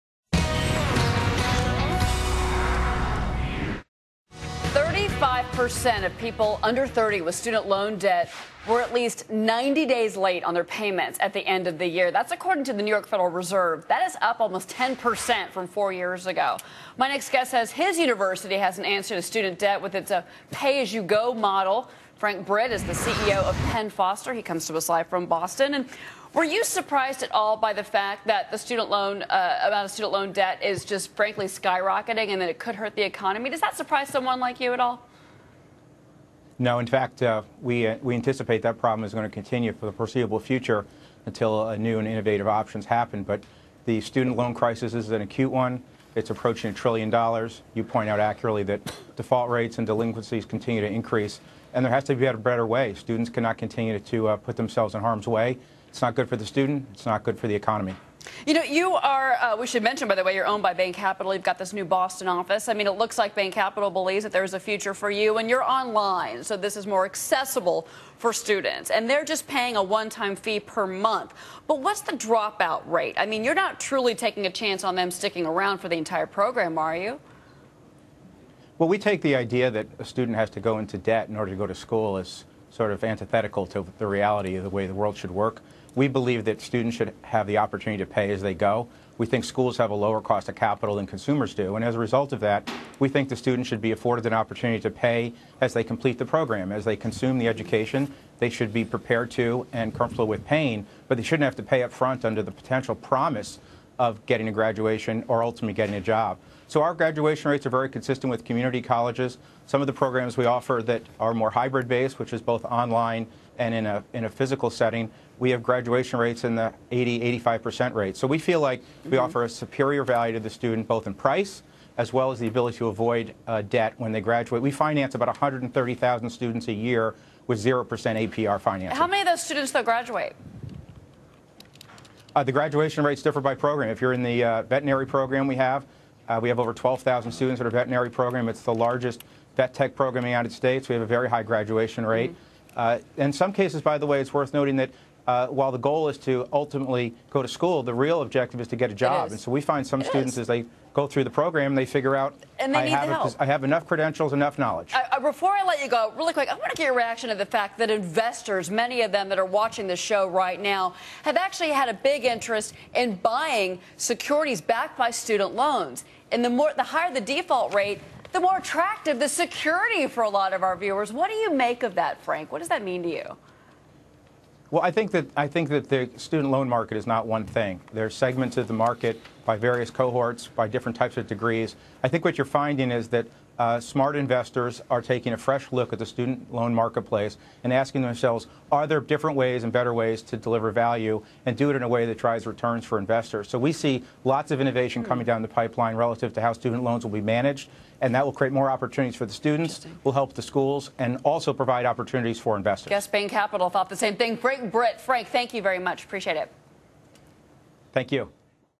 Penn Foster Online Lets Students Pay For Education Monthly
Frank Britt, ceo of Penn Foster, was interviewed on Fox Business News to speak about increasing student loan debt.   "The prospect of having student loans and building debt is a big challenge for students today which is why we continue to focus on debt-free, pay-as-you-go programs that make education accessible to those with less financial means"  Penn Foster College is an online school where students are able to study at their own pace and pay their tuition in low monthly payments.